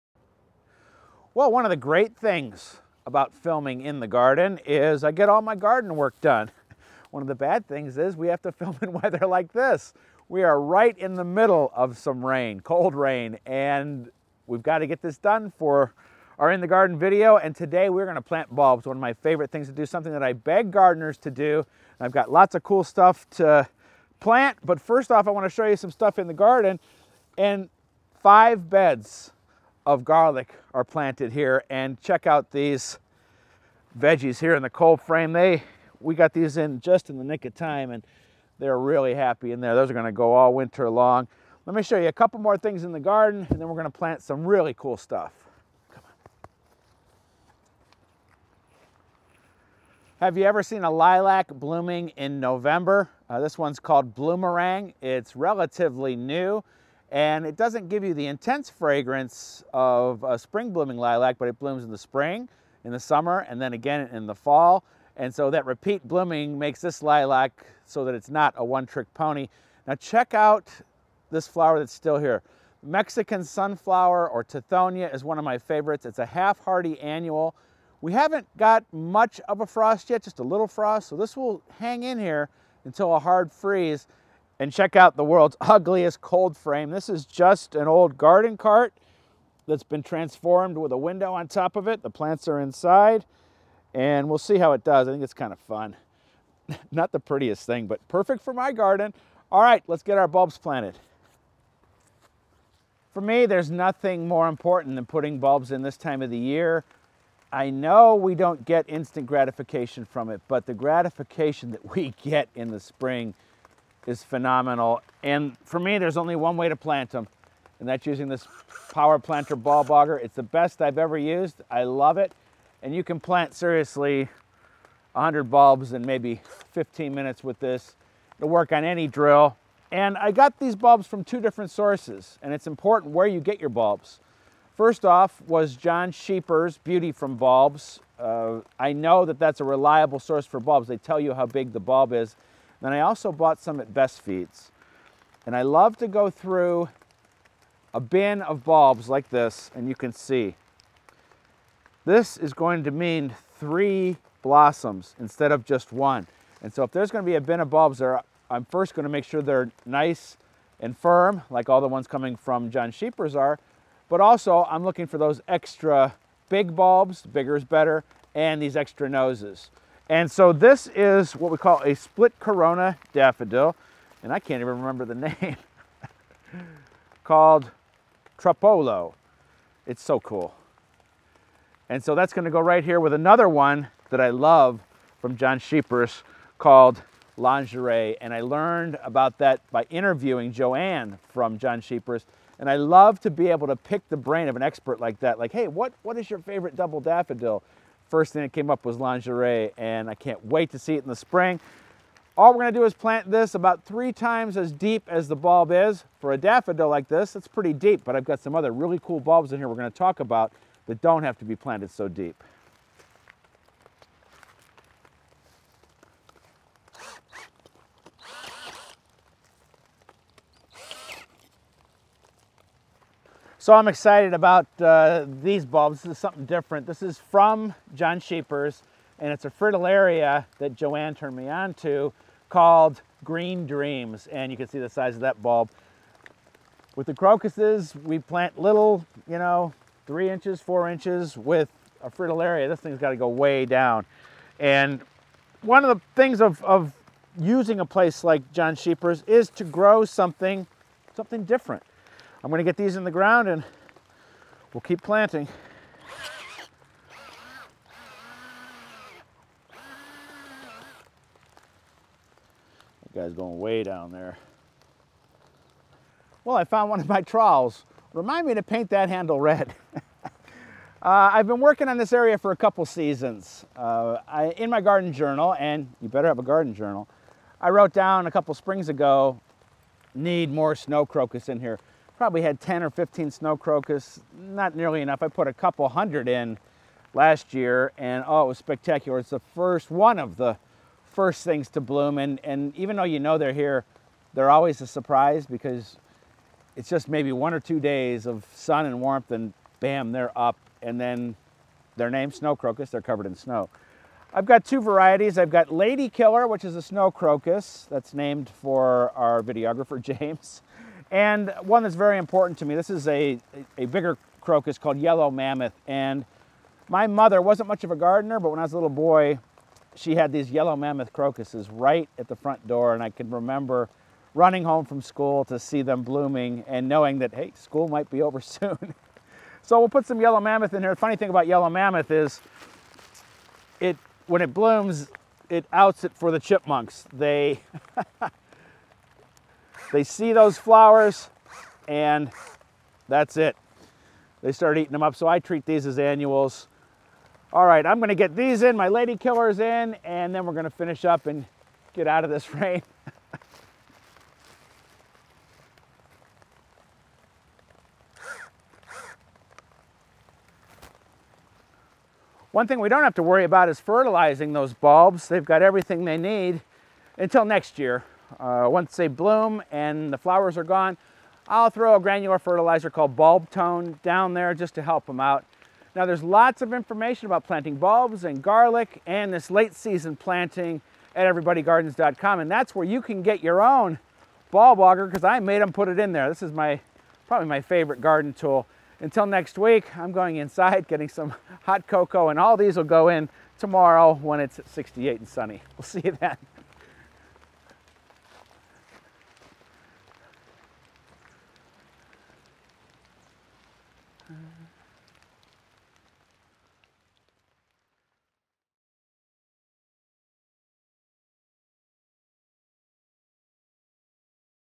In the Garden: A November lilac and how to plant bulbs now for spring beauty
Everybody Gardens editor Doug Oster braves the rain to show off a lilac blooming in November, a Mexican sunflower still going strong and then plants some very cool bulbs.